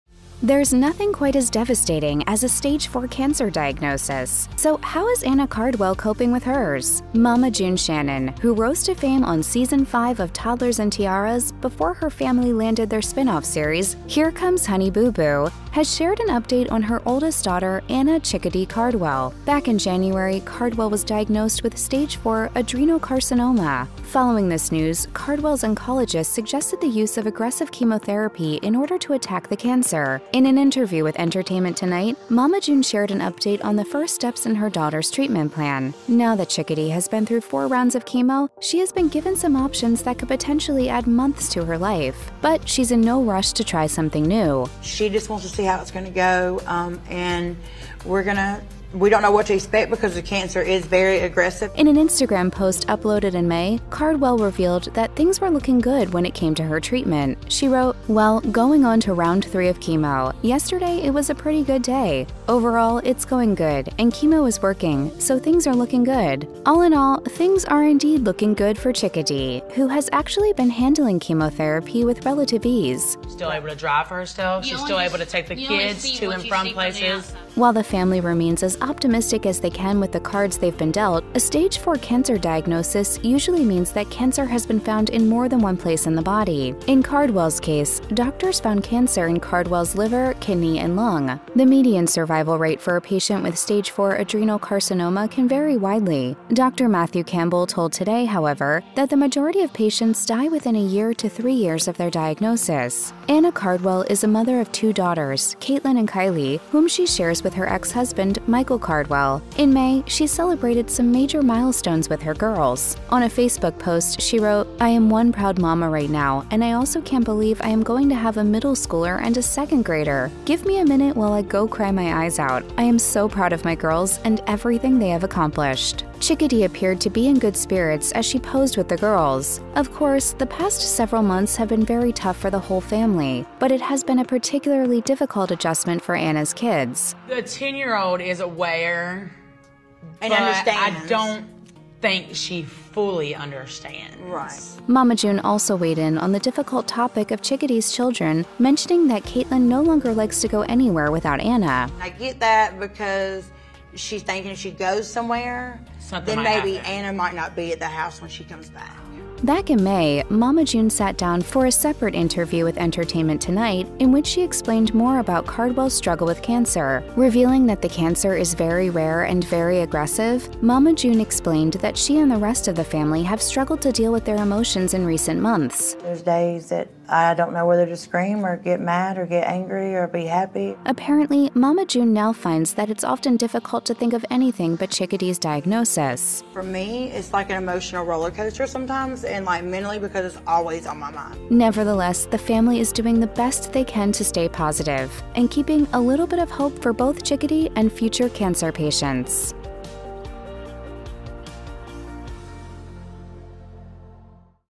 Mama June Reveals Devastating Update On Anna's Health Condition
There's nothing quite as devastating as a stage IV cancer diagnosis — so how is Anna Cardwell coping with hers?